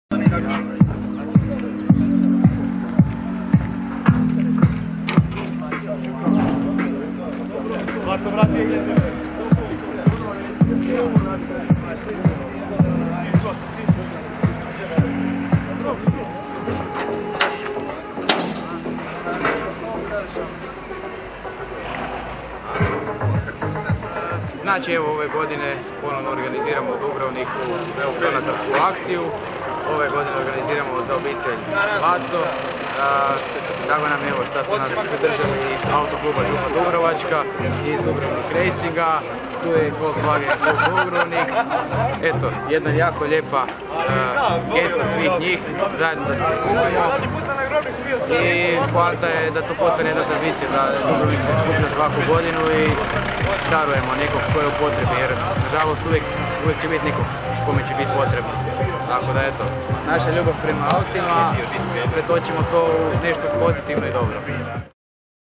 Donatorska akcija za obitelj Laco
U poslovnoj zoni Čibača Mazda Team Croatia danas je organizirao druženje ljubitelja automobila i i donatorska akciju koja će uljepšali Božić obitelji Laco iz Župe dubrovačke.